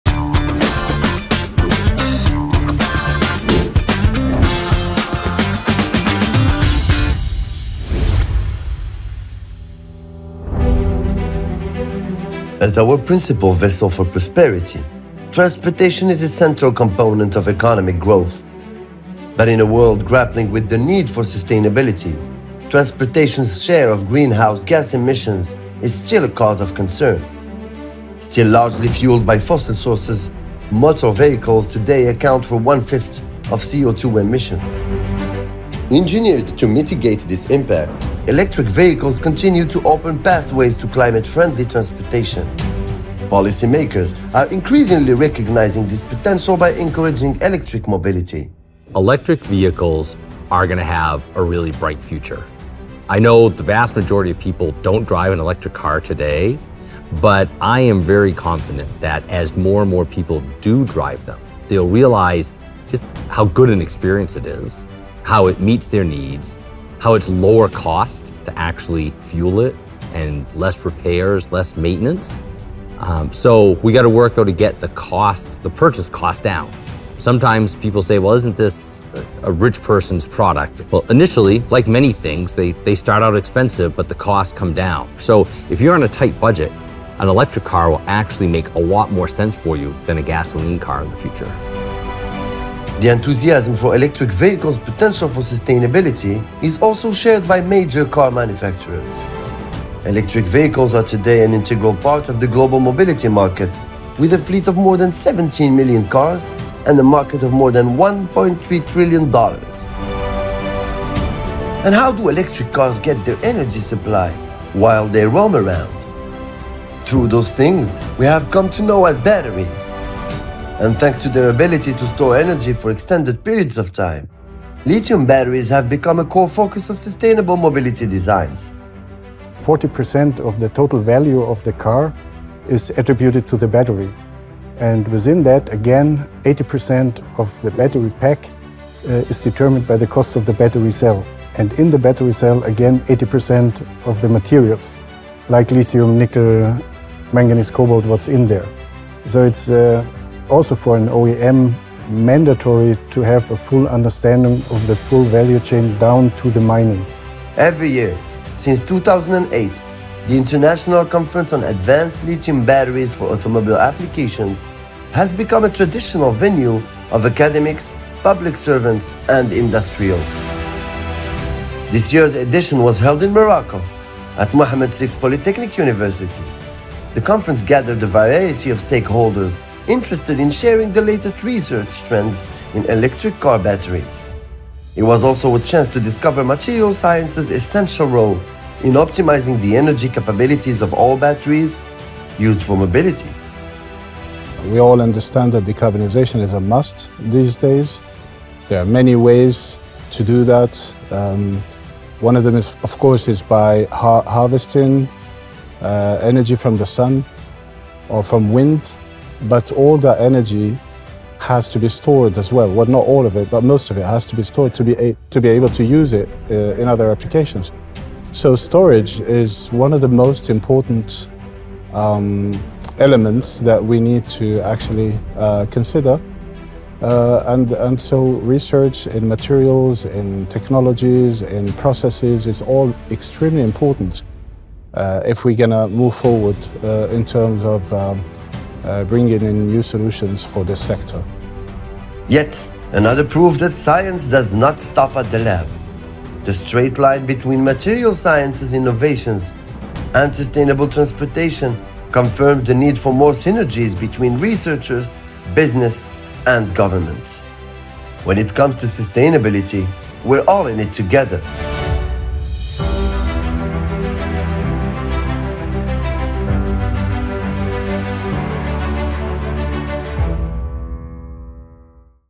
Now You Know #001 - Advanced Lithium Batteries for Automobile Applications (ABAA-13)
Across the globe, transportation fuels our economic growth.

Because of a legacy of reliance on fossil fuels, modern transportation has also been a major contributor to greenhouse gas emissions. Now more than ever, sustainability is front and center for the sector’s supply chains.

Since their inception, electric vehicles have provided a viable alternative in that regard.

Thanks to their role in energy storage, electric car batteries have lately caught considerable attention. Advances in materials science have especially been of great impact in optimizing the performance and lifespan of batteries used for mobility.

At UM6P’s Materials Science, Energy & Nanoengineering Department (MSN), our forward-looking community of researchers continues to explore the ability of science to enhance battery technology. A milestone was reached in that endeavor this month, through the organization of the 13th International Conference on Advanced Lithium Batteries for Automobile Applications at UM6P’s Benguerir campus.

In this first episode of NowYouKnow, we dive deeper into the potential of materials research in catalyzing sustainable transportation; through the eyes of policymakers, car manufacturers and academics.